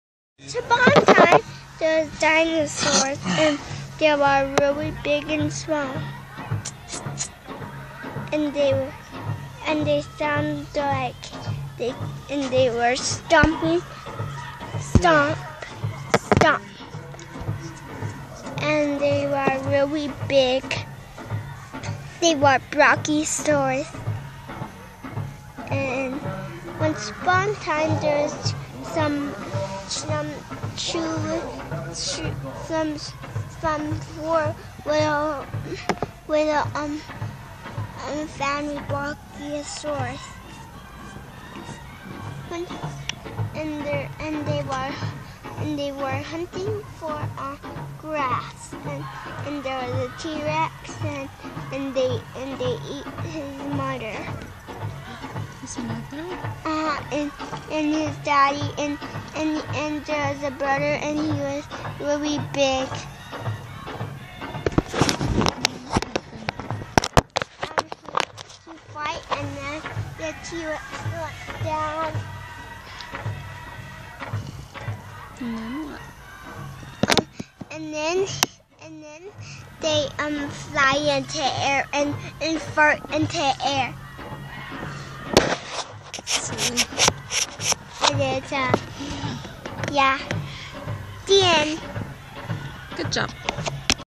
Denzel
Denzel  telling mommy a bedtime story  at 2015 crow fair